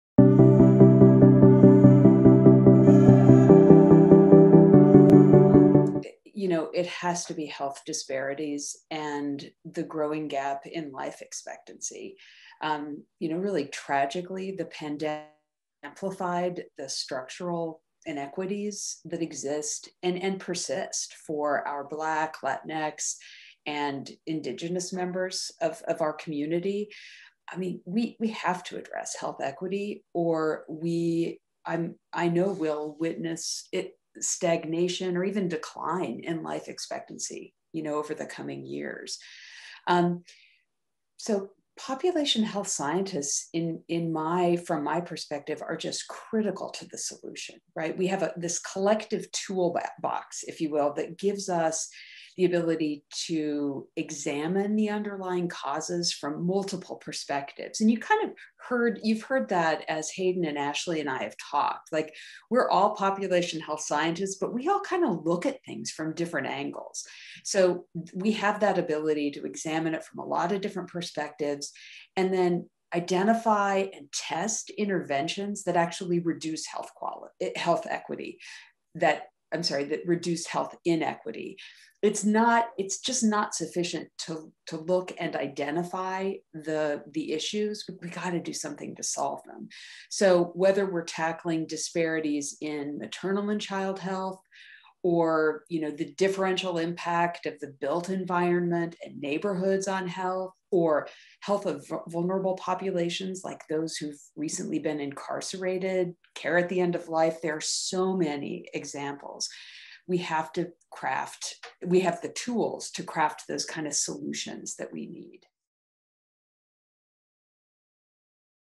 What Are Today's Most Pressing Public Health Issues? | Beginners Guide to Population Health
What are the top public health challenges we face as a nation today, not including the COVID-19 pandemic? And how do you think population health scientists are making a difference?

During the live, virtual event Beginners Guide to Population Health, which was moderated by current M.S. student Aikya Soni, we asked faculty from Duke’s Department of Population Health Sciences this exact question and more. Panelists included:

- Leslie Curtis, Ph.D., Professor & Chair, Population Health Sciences
- Hayden Bosworth, Ph.D., Professor & Vice-Chair, Education
- Asheley Skinner, Ph.D., Professor & Director, Graduate Studies

During the virtual event, faculty members:

- Broke down the basics of population health and what sets it apart from related fields
- Helped attendees understand how population health scientists tackle some of today’s toughest public health challenges
- Familiarized attendees with the tools that population health scientists use to address big healthcare problems that matter
- Inspired viewers with examples of the impact that population health sciences can have on communities and populations around the world